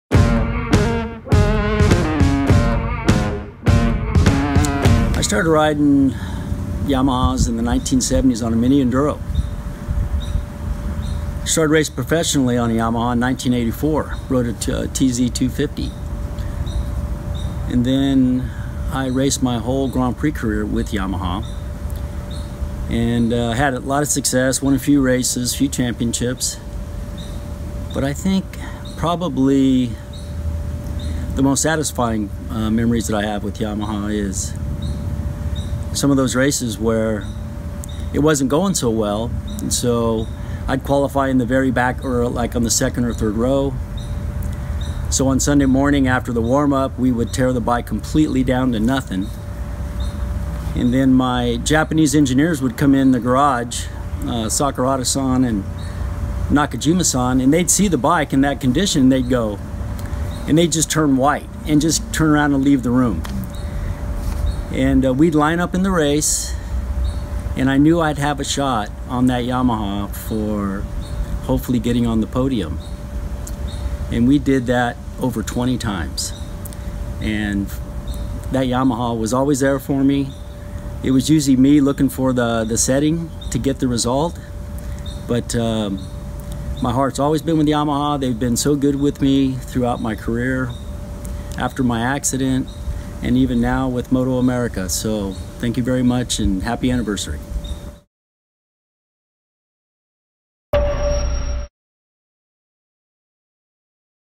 My Favorite Yamaha Memory: Wayne Rainey
Looking back at his fondest Yamaha rides, Wayne Rainey is a Grand Prix Yamaha legend. With 24 wins, 65 podiums and 16 pole positions, Rainey still ranks as one of the most successful American riders in the world.

Life can have many exciting, memorable experiences. Creating opportunities for them is what Yamaha is all about.